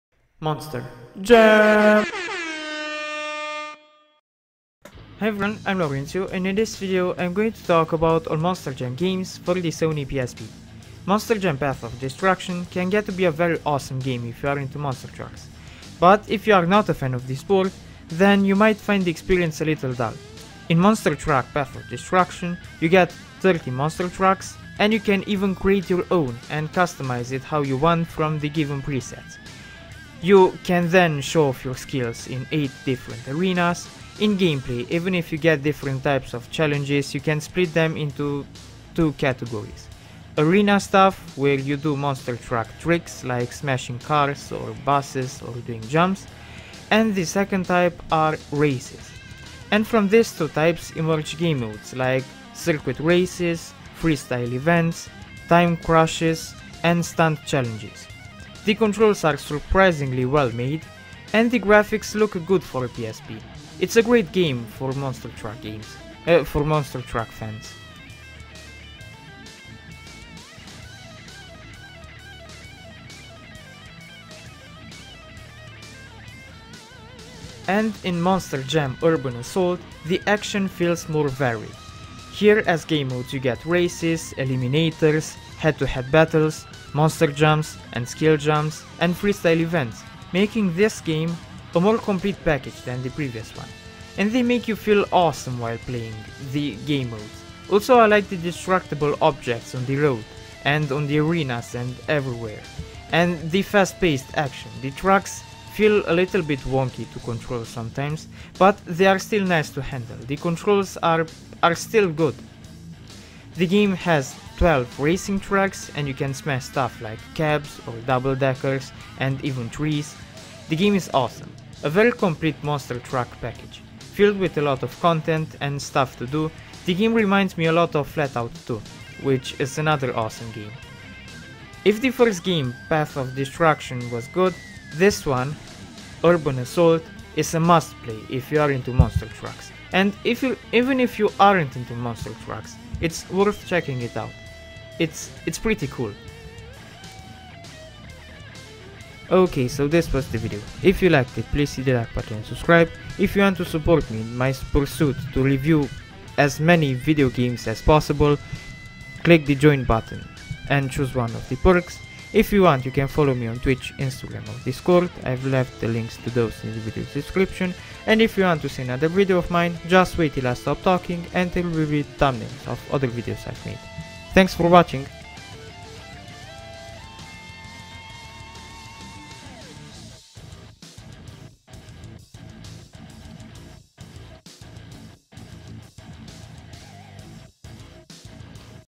All Monster Jam Games for PSP Review
Here is a list of all Monster Jam Games for PSP:
1. Monster Jam: Path of Destruction (2010)
2. Monster Jam: Urban Assault (2008)